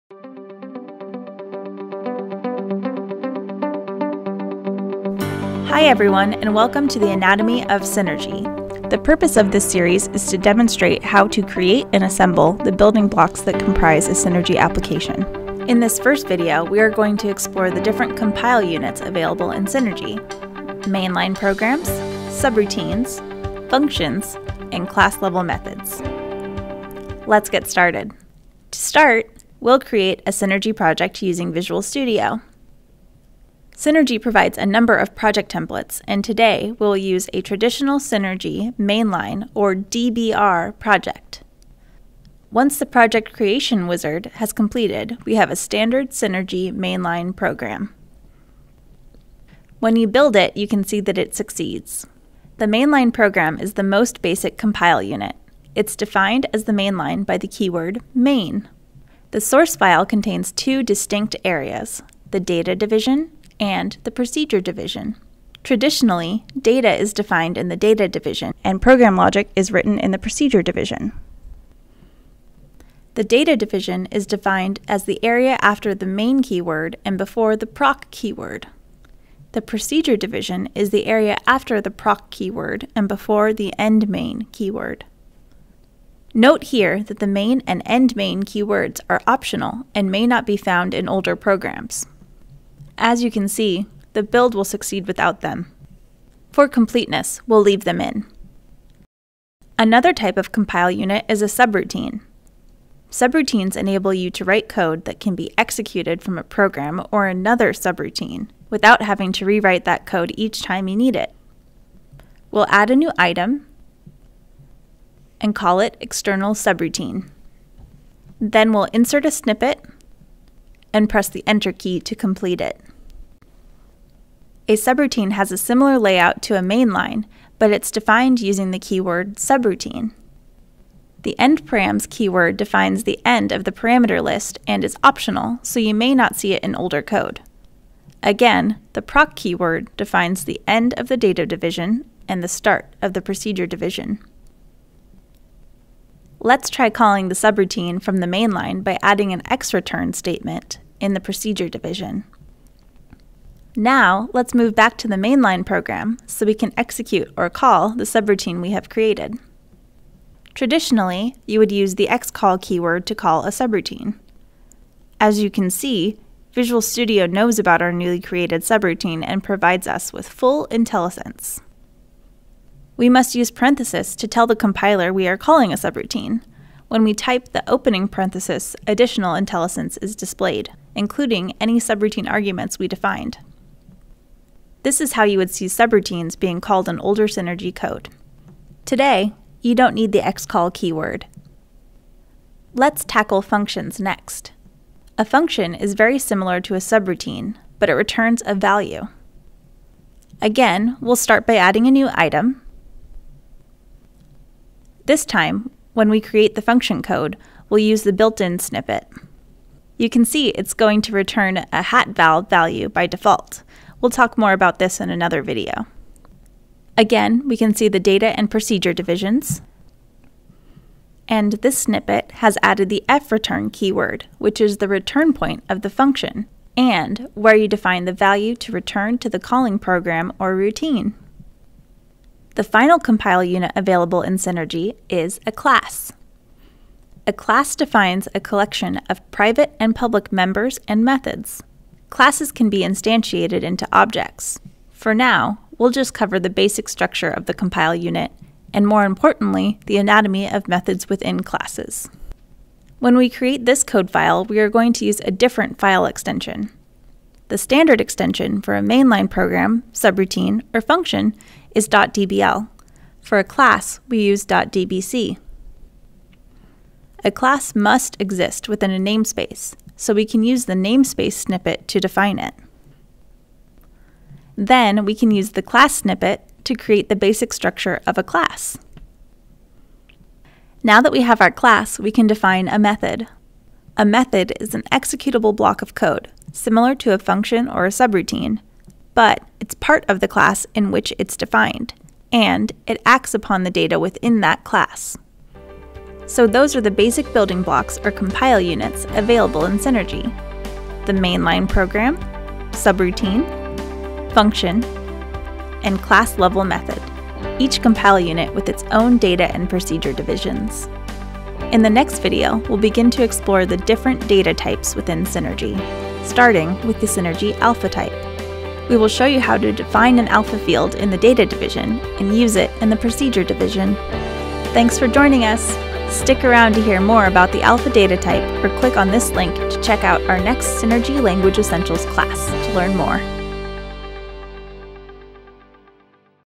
Compile Units | Anatomy of Synergy 1
Are you a new Synergy programmer? Check out this video to learn about the basic compile units of a Synergy program. We'll cover mainline programs, subroutines 1:40, functions 3:15, and classes 3:55.